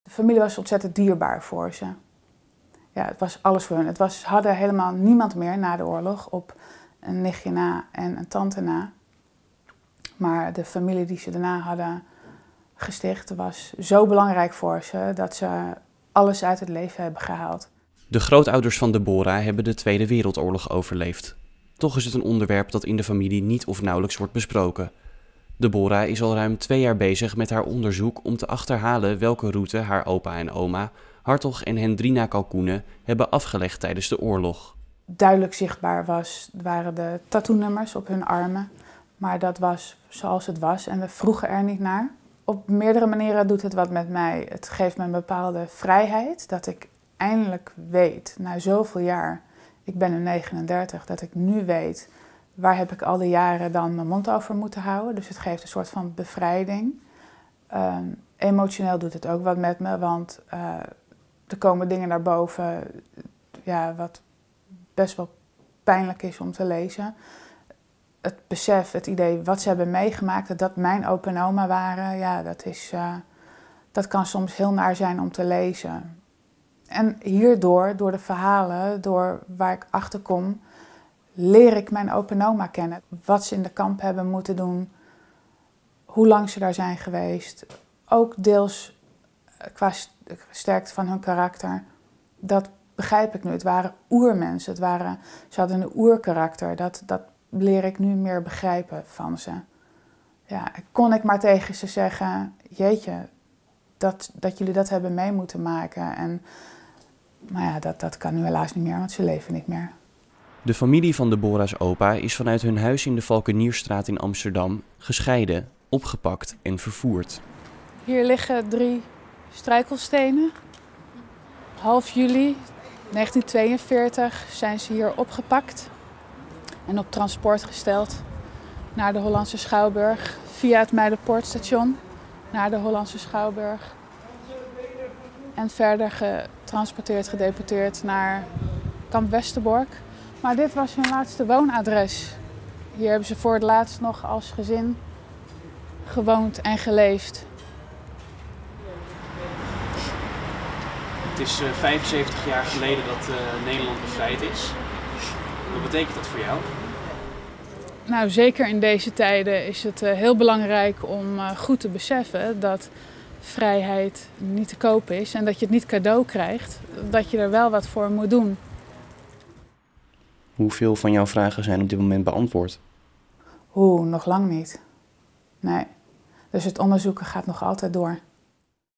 Deborah op zoek naar geschiedenis opa en oma: 'Op meerdere manieren doet het wat met mij'
AMSTELVEEN - Deborah Kalkoene (39) uit Amstelveen is al zo'n twee jaar bezig met onderzoek naar hoe haar grootouders de Tweede Wereldoorlog hebben ervaren. Ze heeft hier nooit met hen over gesproken. Via online en fysieke archieven probeert ze nu de Joodse geschiedenis van opa en oma in oorlogstijd vast te leggen.

Hartog en Hendrina Kalkoene leerden elkaar na de oorlog kennen, in betere tijden. Beiden overleefden die gruwelijke periode. Hartog werd naar meerdere werk- en concentratiekampen vervoerd, maar opa heeft daar nooit een woord met zijn kleindochter over gerept.

Pijnlijk

Daarom is Deborah zelf op onderzoek uitgegaan. In het begin kon ze dagen en nachten achter elkaar blijven zoeken op het internet. ,,Emotioneel doet dat ook wat met me, want er komen dingen naar boven die best pijnlijk zijn om te lezen. Het besef, het idee wat ze hebben meegemaakt, dat dat mijn opa en oma waren, dat kan soms heel naar zijn om te lezen."

Door in hun levensgeschiedenis te duiken en de moeilijke waarheden onder ogen te zien, begrijpt Deborah haar grootouders een stuk beter. "Het waren oermensen. Ze hadden een oerkarakter. Dat leer ik nu beter te begrijpen."

De opa en oma van Deborah zijn respectievelijk in 2010 en 2015 overleden. "Kon ik maar tegen ze zeggen: jeetje, dat jullie dat hebben mee moeten maken. Dat kan nu helaas niet meer."

Laatste woonadres

In juli 1942 woonde Hartog Kalkoene samen met zijn broertje en ouders in de Amsterdamse Valckenierstraat. "Dit was hun laatste woonadres. Hier hebben ze voor het laatst als gezin gewoond en geleefd." Het gezin werd in juli van dat jaar wreed uit elkaar gerukt. Zijn broertje en ouders werden opgepakt en vanuit de Hollandsche Schouwburg gedeporteerd naar kamp Westerbork. Hartog was toen hoogstwaarschijnlijk aan het werk maar werd  anderhalf jaar later alsnog ook opgepakt.

Struikelstenen

De familie heeft voor het huis drie struikelstenen geplaatst als herinnering aan het drama dat zich hier in de vorige eeuw heeft voltrokken. Volgens Deborah is het belangrijk om stil te staan bij de vrijheid die we nu kennen en dat we die niet als vanzelfsprekend moeten zien. "Zeker in deze tijden is het belangrijk om goed te beseffen dat vrijheid niet te koop is en dat je die niet cadeau krijgt, je moet er wel wat voor doen."

Op de vraag of Deborah antwoord heeft gekregen op al haar vragen en of ze haar onderzoek naar haar grootouders kan afronden, is ze kort: "Nog lang niet, nee. Het onderzoeken gaat nog altijd door."

Joodse Huizen

Deborah heeft ook meegewerkt aan het boek 'Joodse Huizen', waarin verhalen van 24 auteurs staan over vooroorlogse bewoners. "Ik heb door mijn onderzoek een mooi verhaal over mijn grootvader kunnen schrijven."